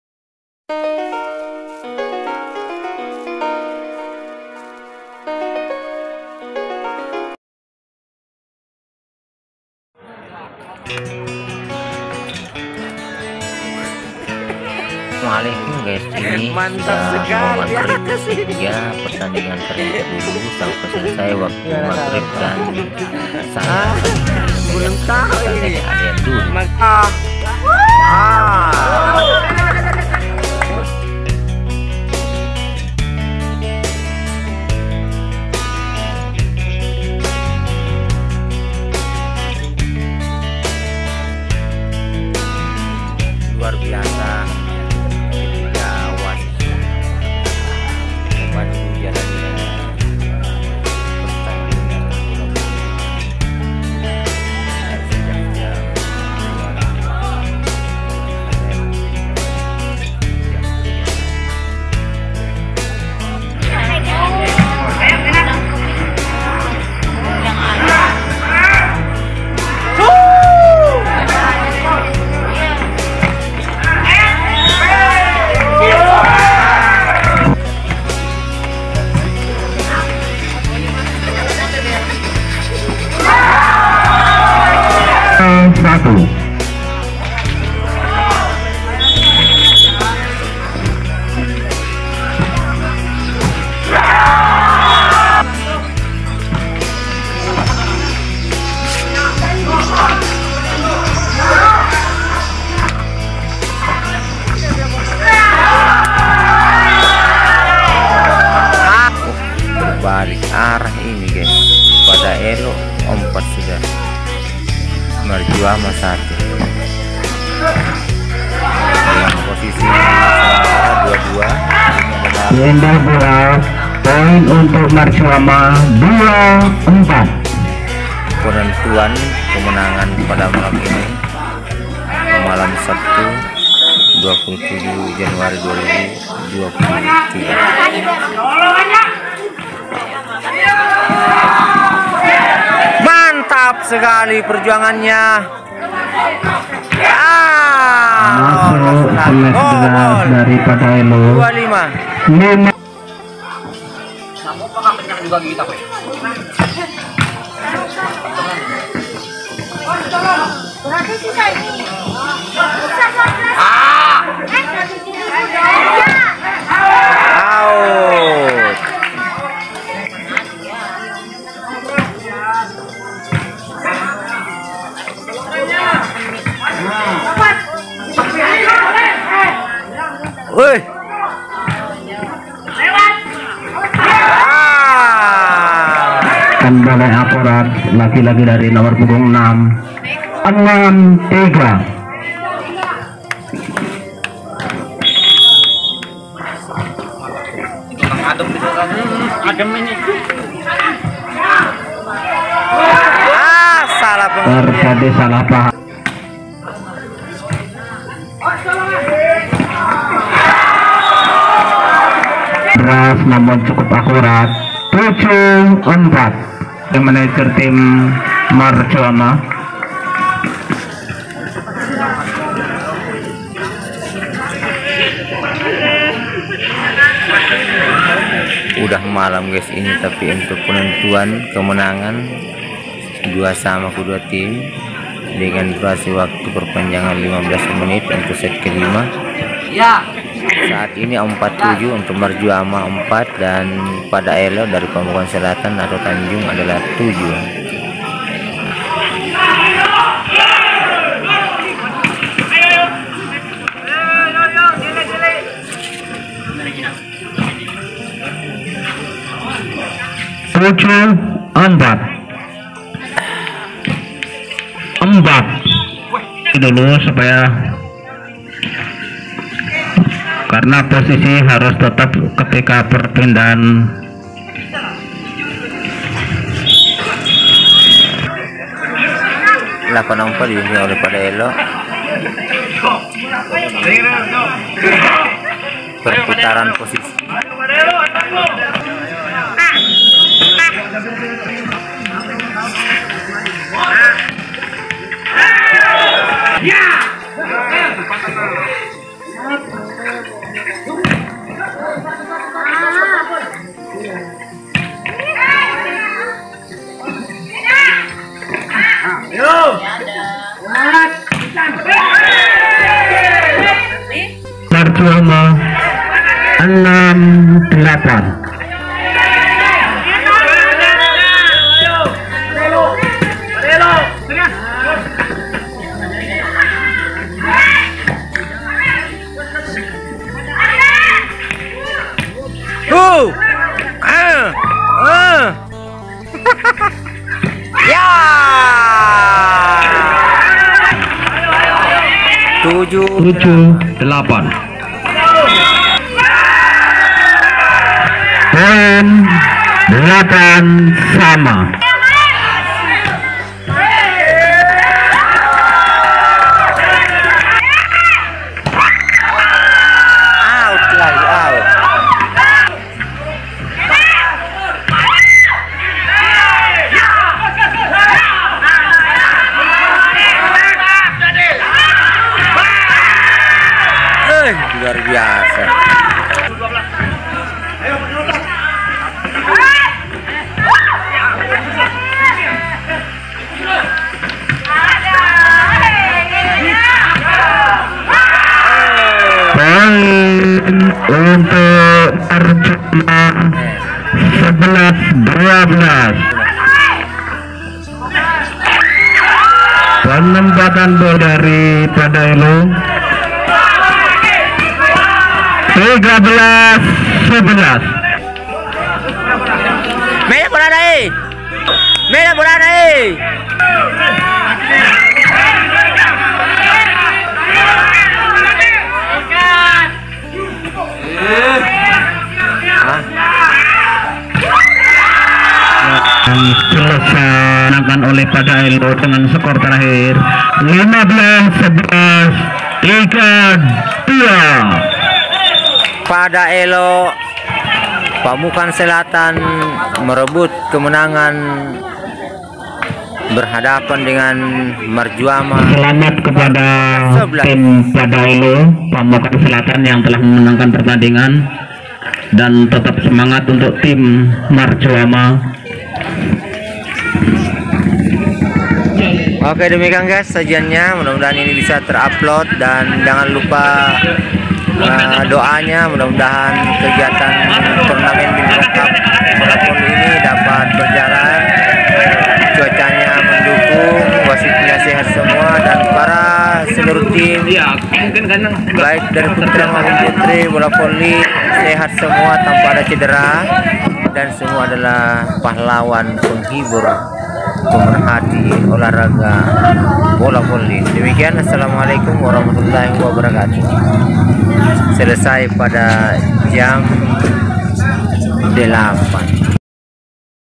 Detik menegangkan Tim Volly Putra Pada Elo Tanjung di Set ke 5 melawan Marjuama
Semakin Seru Lanjutan Volly Putra di Set 5
akhirnya Tim Volly Putra pada Elo membangun seeangan bertubi2 dgn posisi 2-2  dan diakhir set 5  Tim PADA ELO  mampu menuju babak 8 besar.